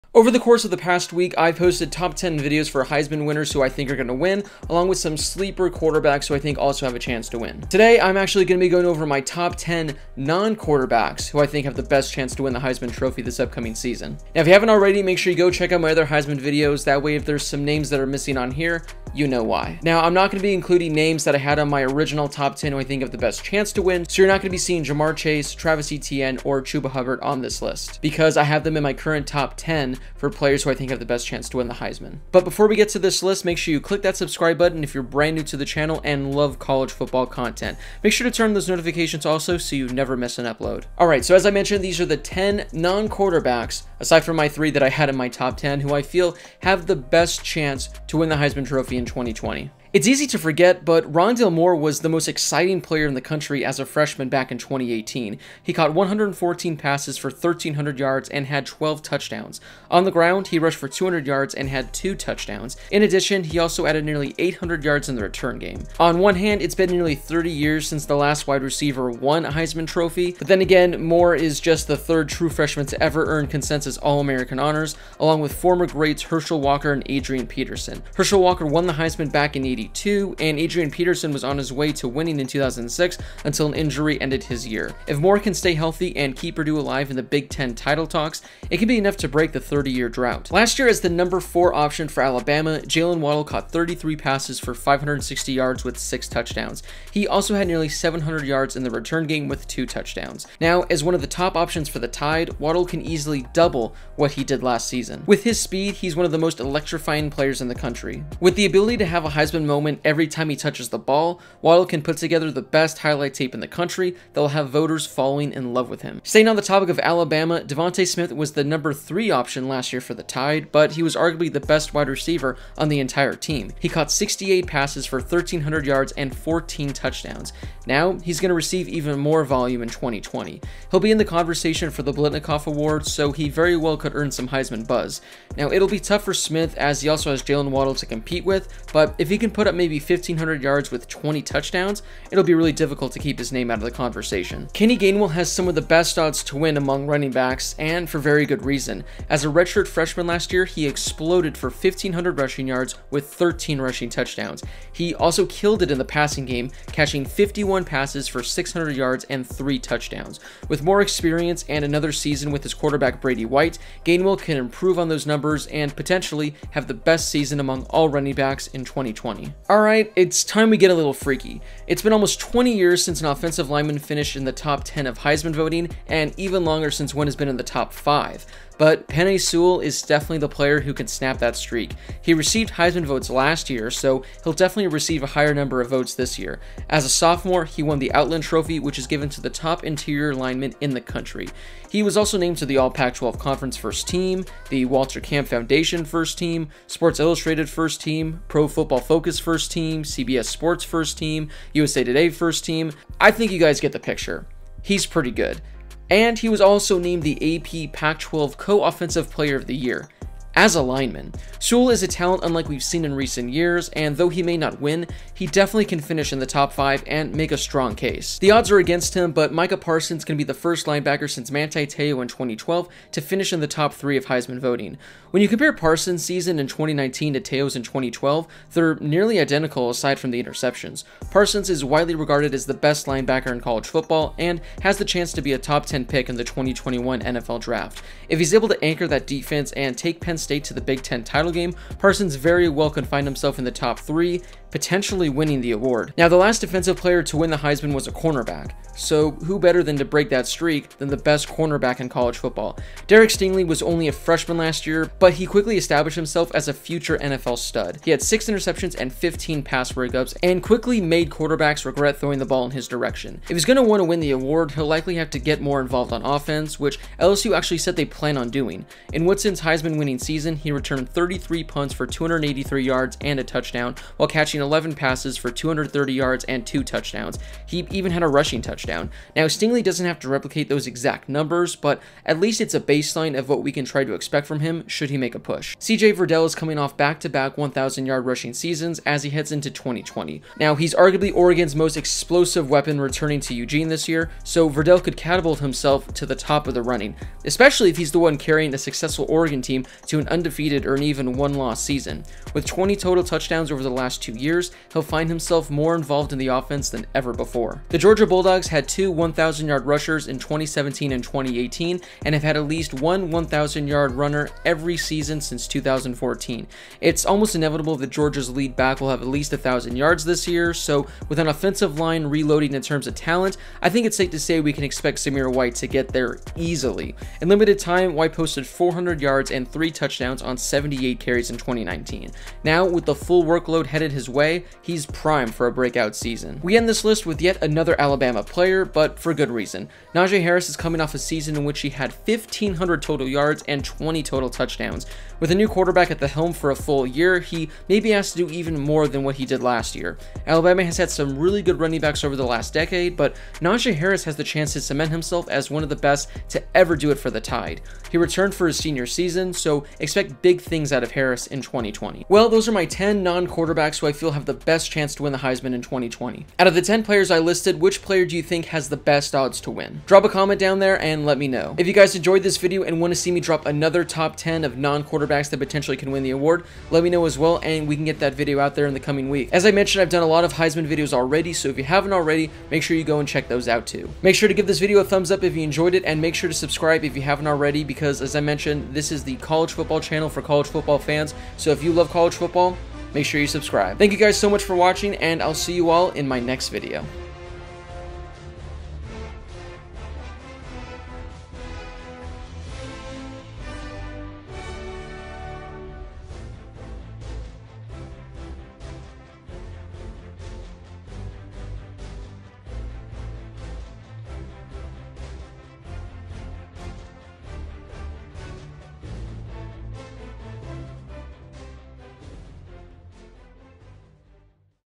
10 NON-QB's who can WIN the Heisman in 2020
The Heisman Trophy has become a quarterback award, sadly. Although it's likely going to go to another QB this year, here are 10 NON-QB's who can potentially win the Heisman in 2020!

If you love college football, this is the channel for you.